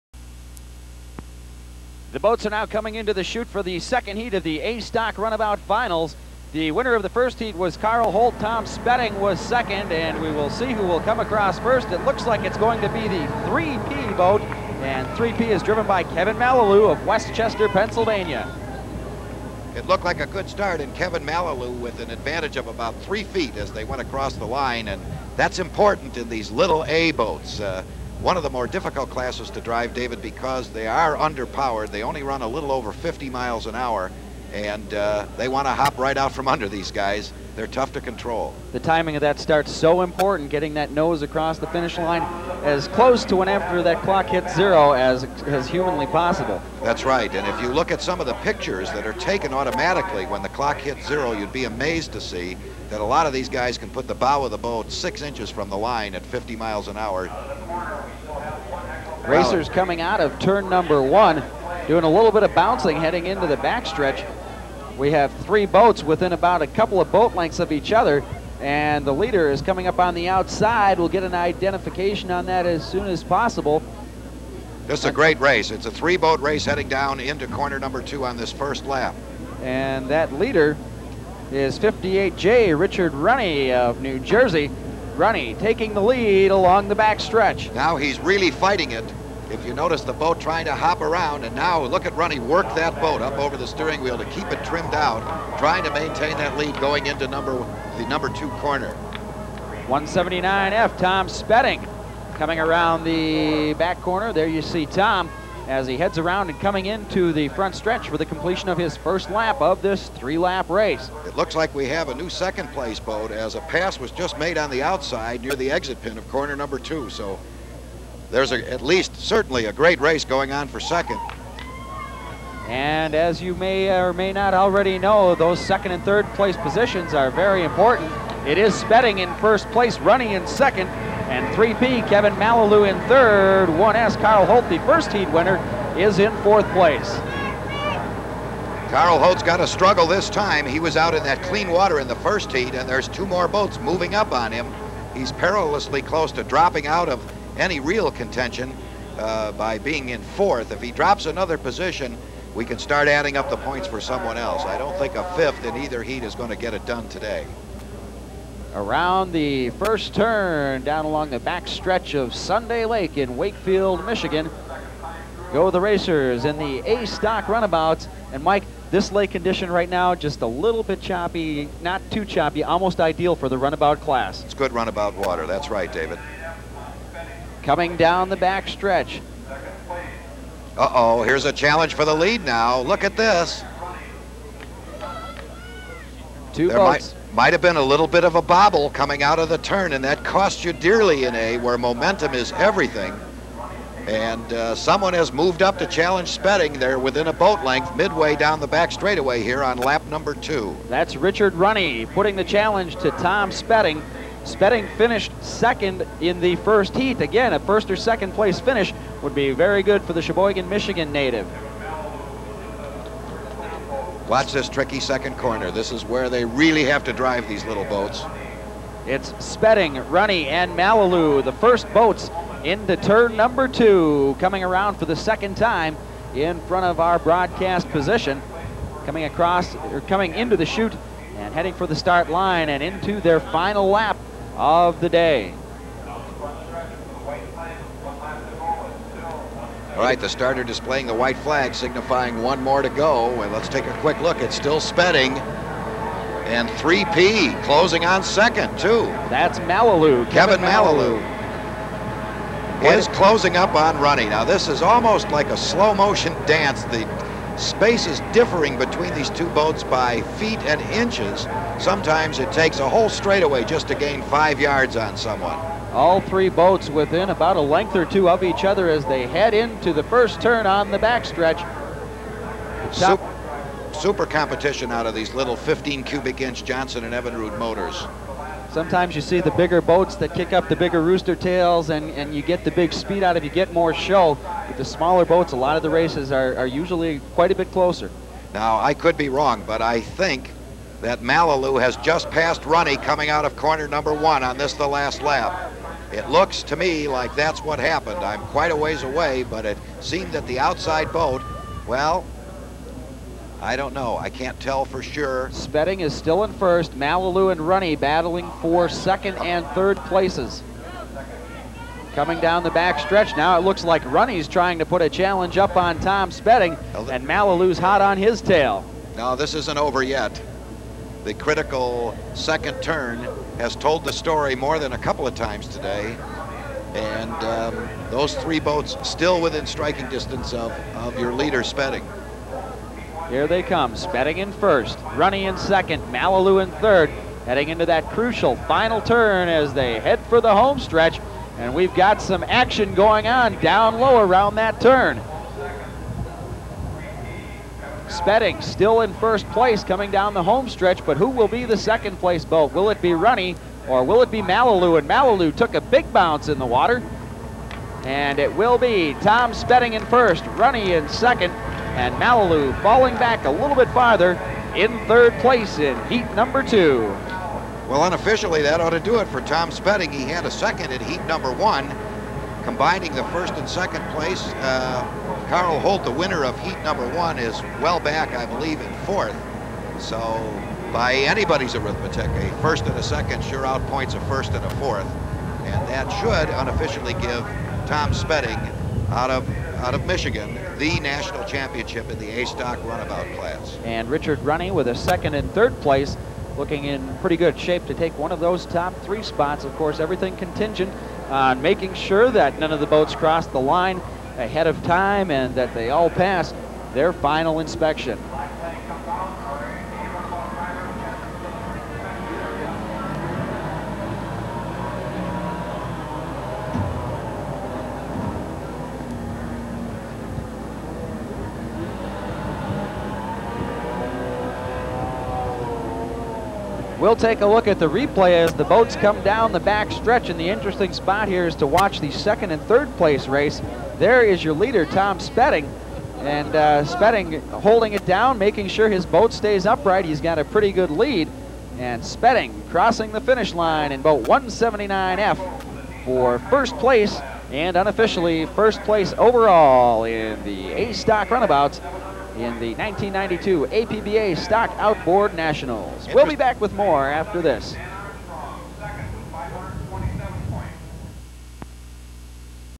1992 APBA Stock Nationals ASR Heat 2
Heat 2 of the 1992 APBA Stock Nationals ASR  finals held in Wakefield, MI.